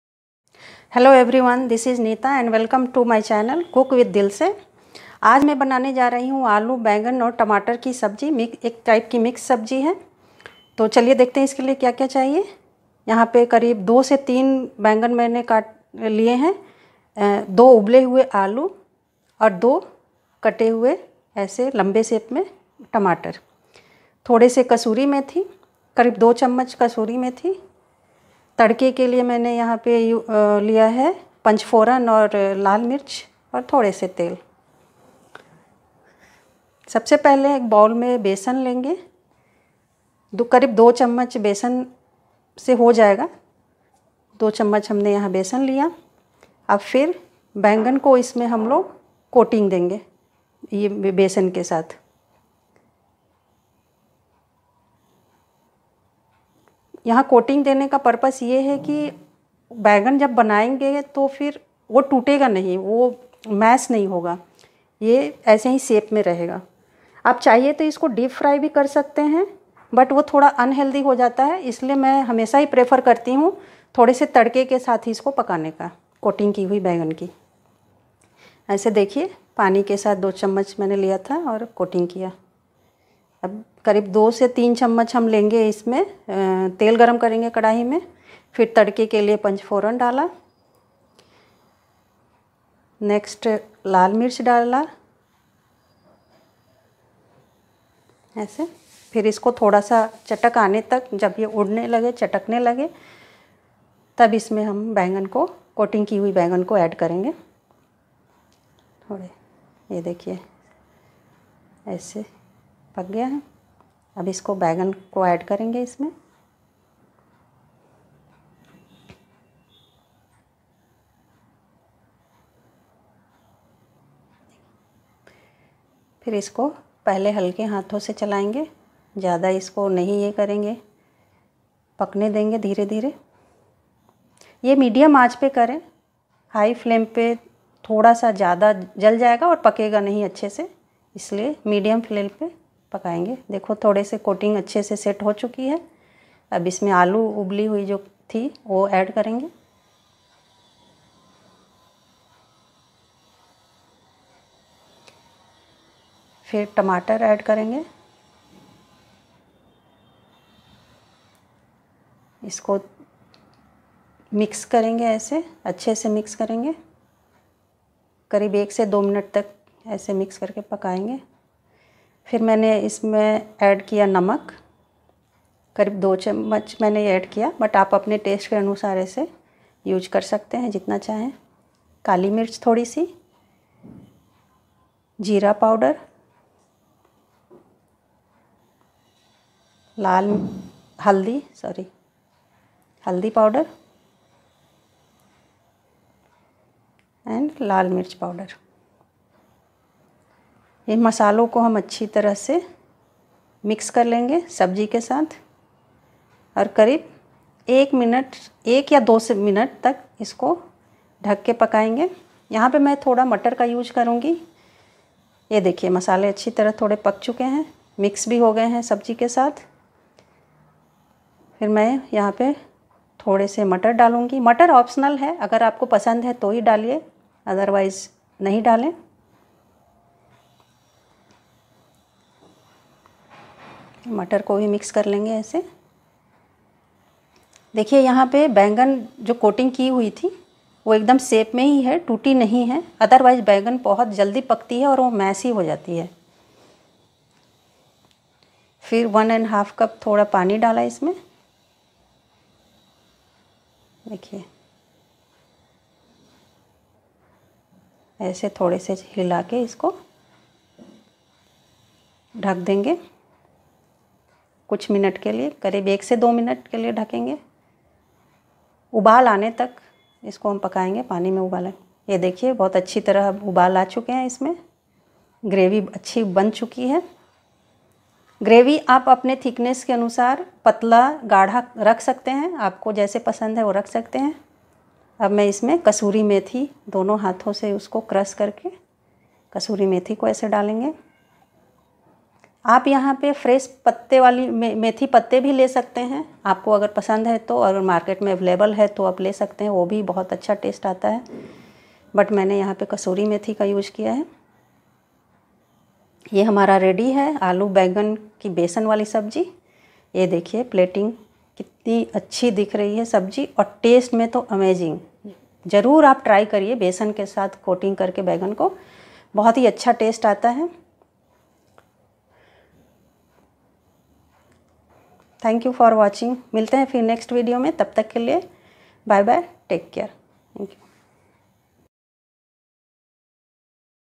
क्या आप इस विधि से बनी हुई सब्जी खाई है|| आलू-बैंगन की बेसन वाली सब्जी ||must try
Must try this recipe, aallo baign ki mix sabji with basan,  Super-duper Yummy.  Must try

Recipe by cook with dil se

aloo baingan ki sabji ki recipe.
aloo baingan ki sabji dhaba style.
aloo baingan ka bharta.
aloo baingan curry recipe.